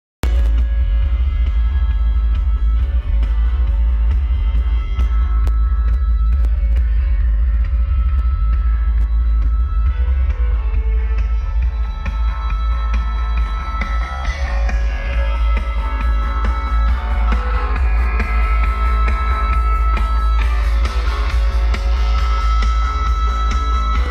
JBL P1222
JBL P1222 bridged on Sound Magus G150 (4x 150wrms 4ohm) sealed box
Front system - Phase CFS